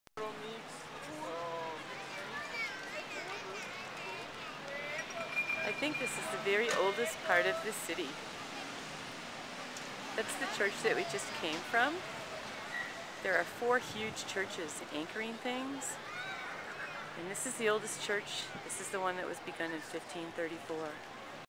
San Francisco Church Quito
The oldest church in South AMerica